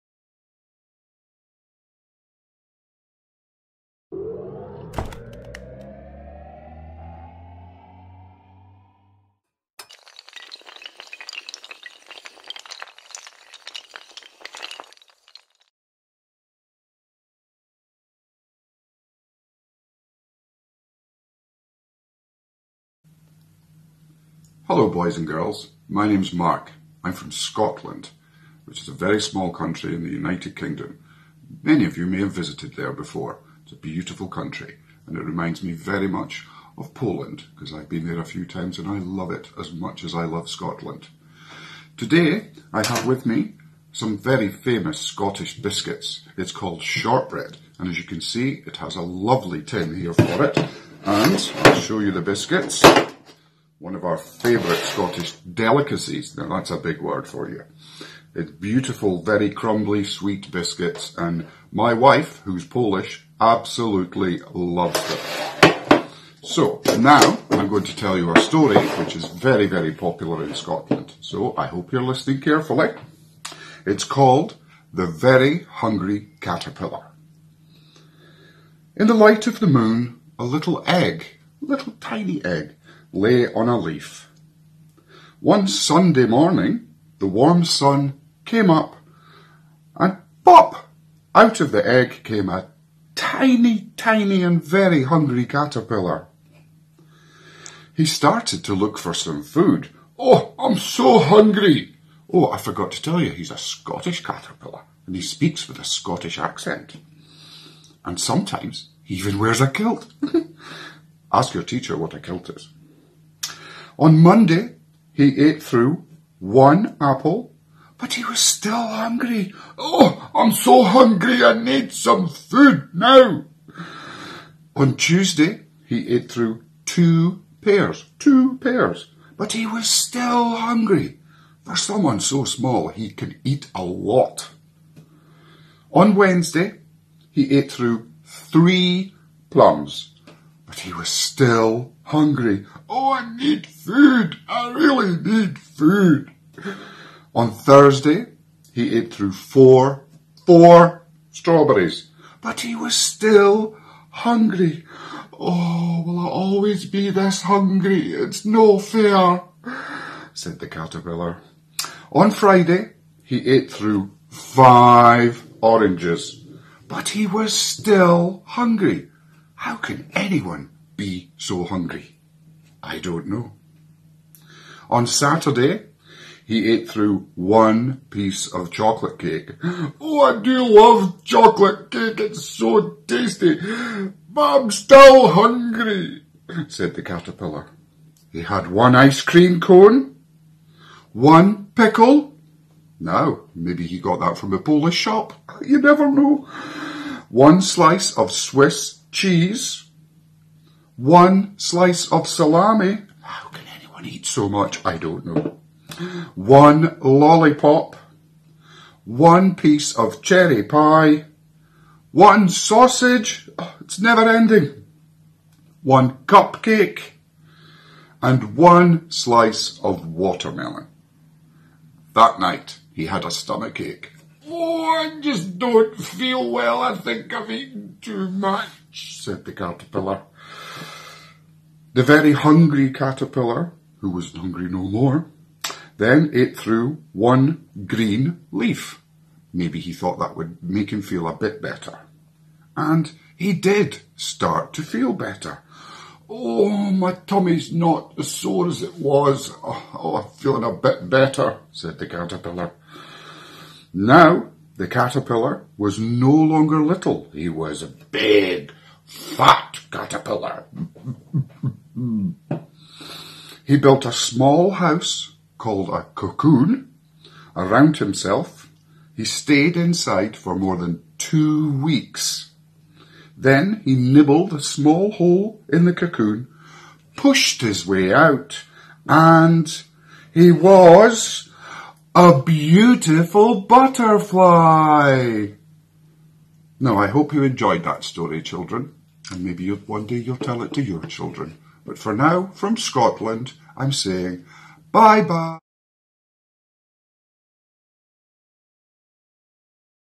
Film nr 135 Mark ze Szkocji
135 film w ramach akcji Zaczytajmy się w Kotanie. Dzisiaj kolejna międzynarodowa bajka dzięki projektą e-learningowym, które odbywają sie w naszej Szkole - tym razem ze Szkocji. Pan Mark przeczyta nam typowo szkocką opowieść o Bardzo głodnej gąsienicy.
Zapraszamy